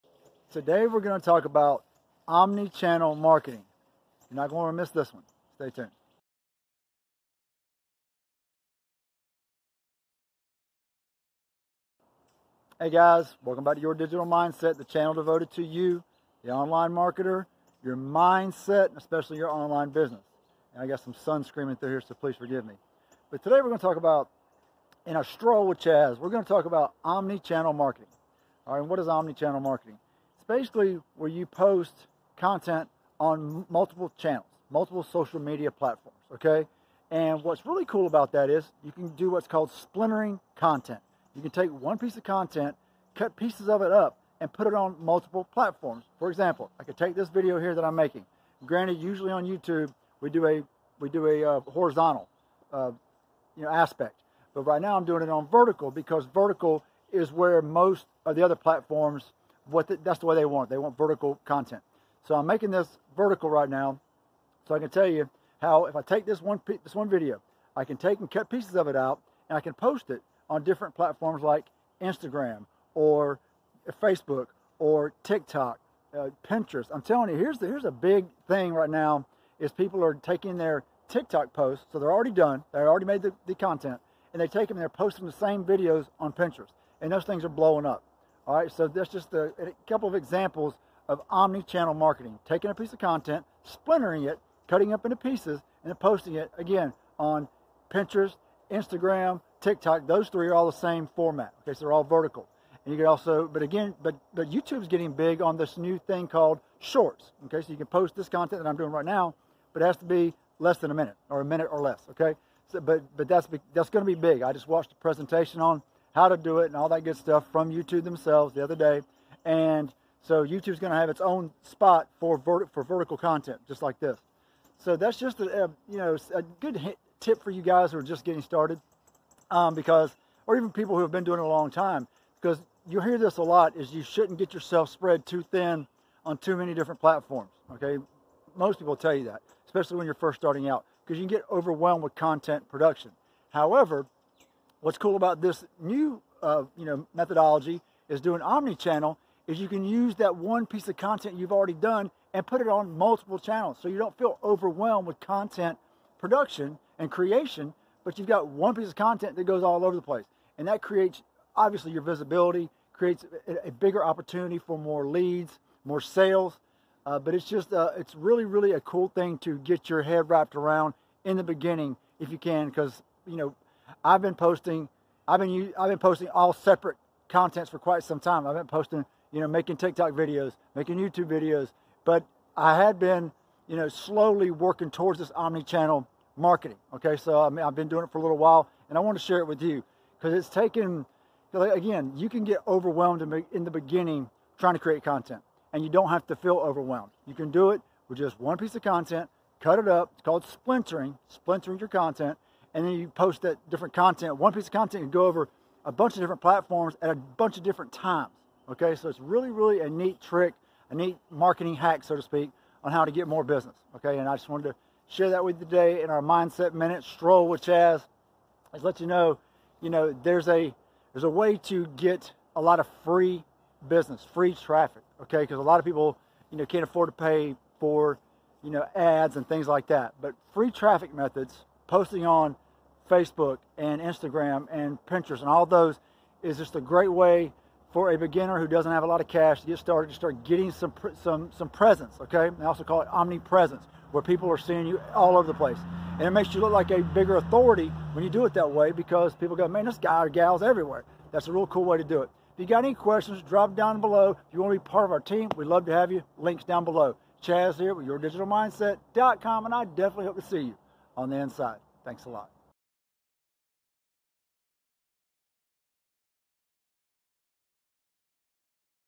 What is OMNICHANNEL MARKETING? - How Can You Use OmniChannel Marketing for Business
What is OMNICHANNEL MARKETING? . . In This Video from YourDigitalMindset.com You'll Discover 'The Truth About OmniChannel Marketing' & Also 'How To Do OmniChannel Marketing The Right Way'. Additionally, You'll Learn a New Term Called 'Splintering'.

Follow 'Your Digital Mindser' on Facebook, Instagram, TikTok, Quora or Wherever You Like To Spend Your Time. WE'RE EVERYWHERE.